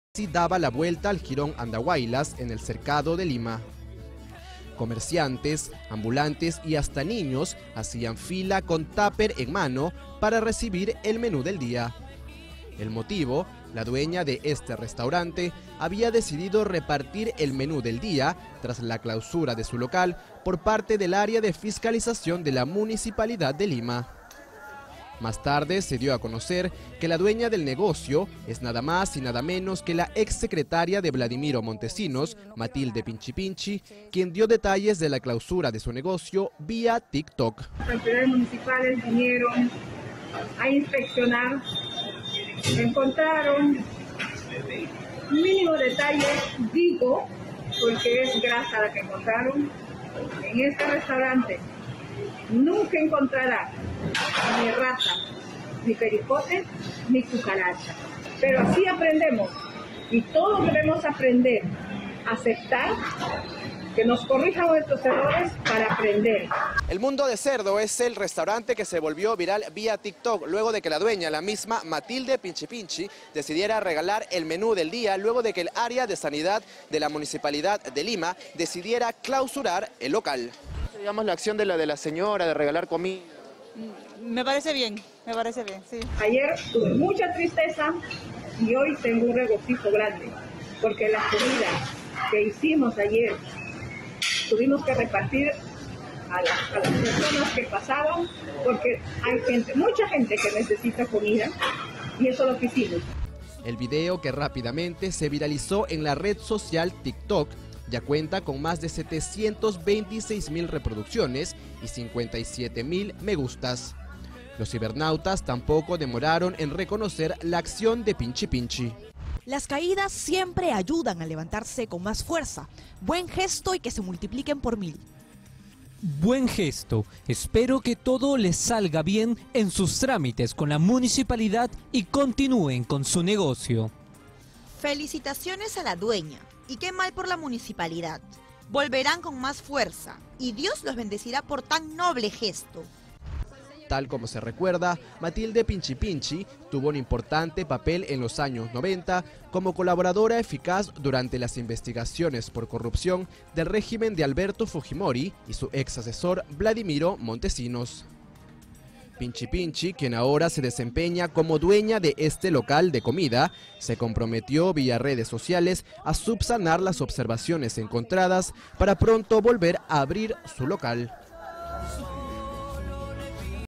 Cercado de Lima: Fiscalizadores clausuran restaurante y dueña regala la comida
A través de redes sociales, un curioso video se habría viralizado, luego que agentes fiscalizadores de la Municipalidad de Lima clausuraran un restaurante, y la dueña decidiera regalar la comida para que no se malogre.

Según se pudo ver en las imágenes compartidas, largas colas se habrían formado en el exterior del restaurante ubicado en jirón Andahuaylas en Cercado de Lima.

En el lugar se pudo apreciar comerciantes de la zona, ambulantes y hasta niños, esperando con un taper para poder recibir un plato de comida gratis.

IDENTIDAD DE LA MUJER

La generosa mujer respondería al nombre de Matilde Pinchi Pinchi, quien habría sido ex asesora de Vladimiro Montesinos en el régimen del ex mandatario Alberto Fujimori.

Asimismo, se sabe que Pinchi Pinchi habría compartido el clip a través de su red social Tik Tok, alcanzando 726 mil reproducciones y 57 mil me gusta.